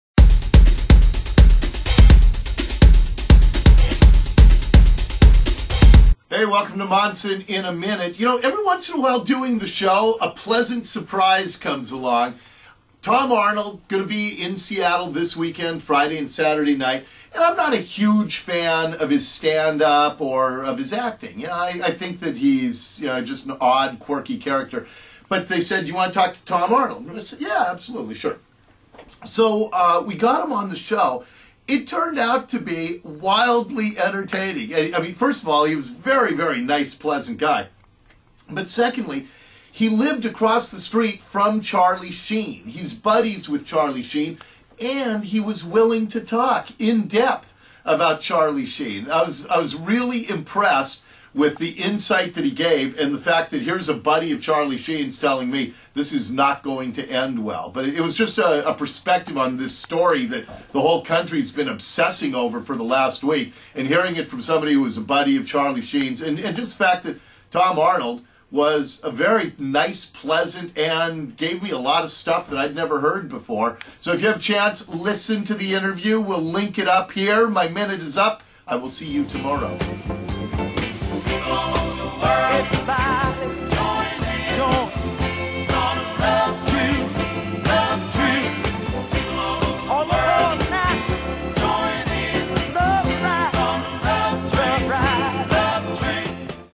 Monson in a Minute - 03/03/2011
Tom Arnold dishes on Charlie Sheen,